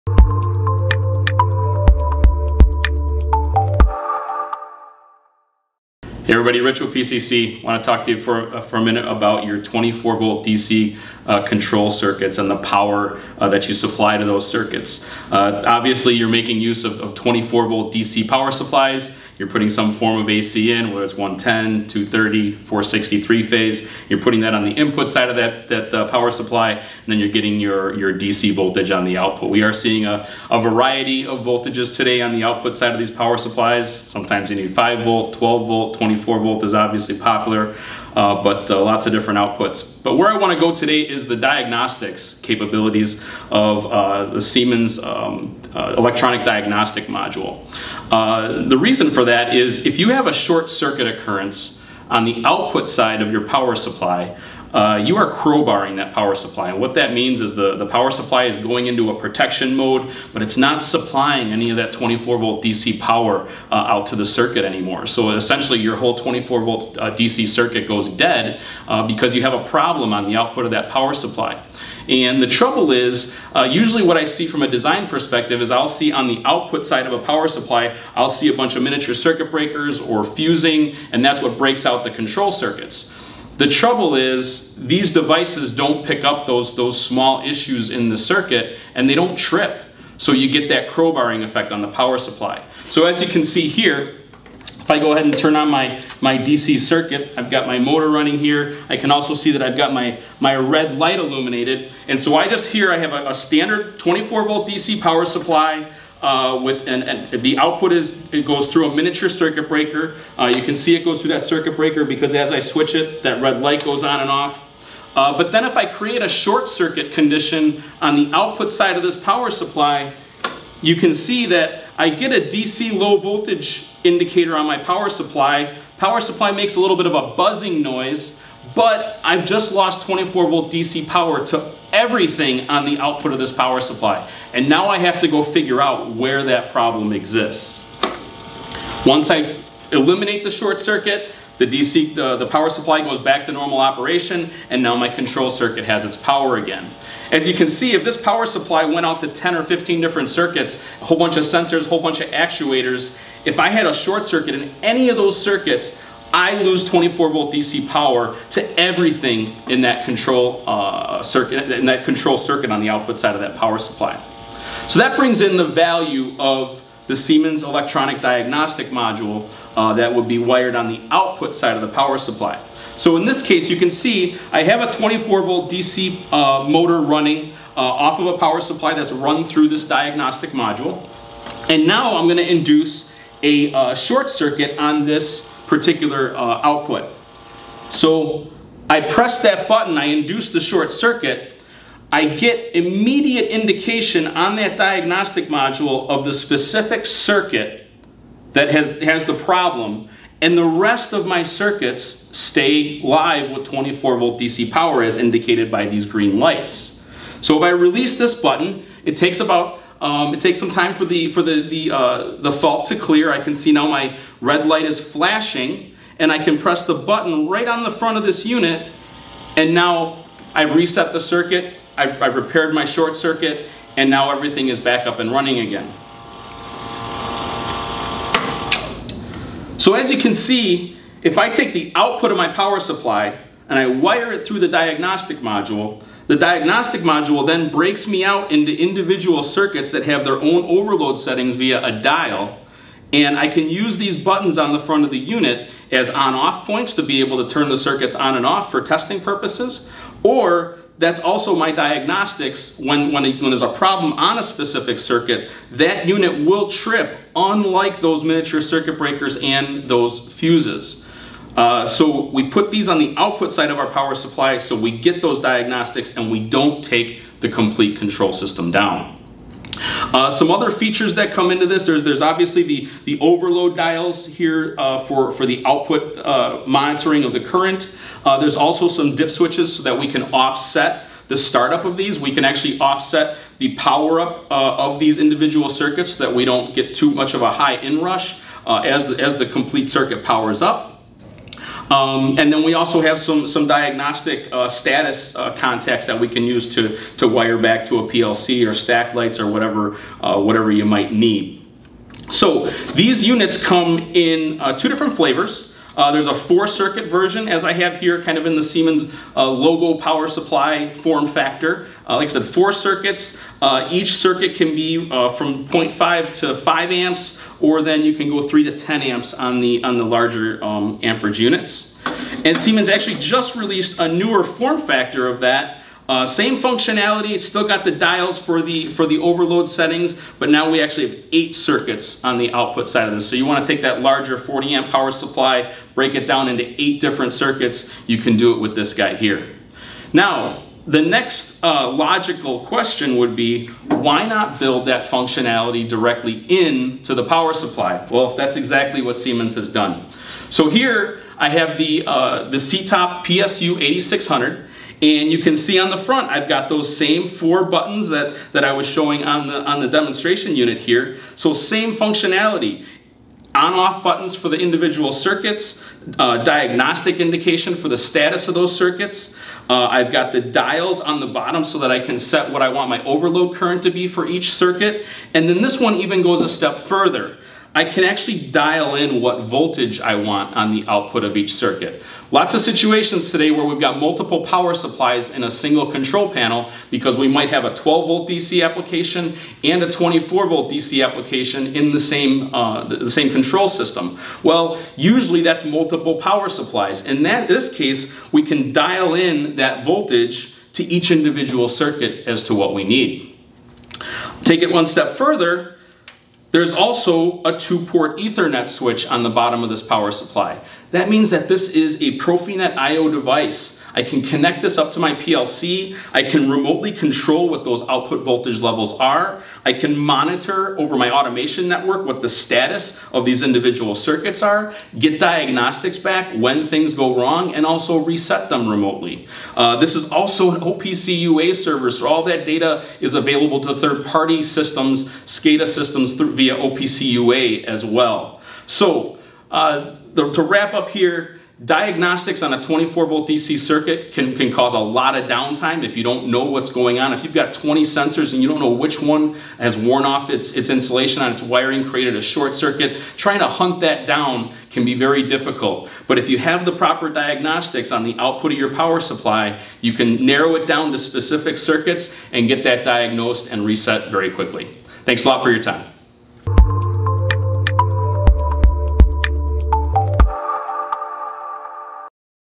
Siemens SITOP Selectivity & Diagnostic Module | PCC Straight to the Point
Have you ever encountered a crowbarred power supply? All it takes is one single fault and you can lose your control power to a complete system. Fuses and circuit breakers will NOT prevent this situation. The SITOP selectivity and diagnostics modules are the device you need on the output of all DC power supplies. They distribute and monitor the load current across many circuits. Overloads and short circuits are reliably detected and the faulty circuit disconnected, all while the other loads continue uninterrupted. Check out this video where Rich discusses this topic in further detail and demonstrates the functionality of these devices.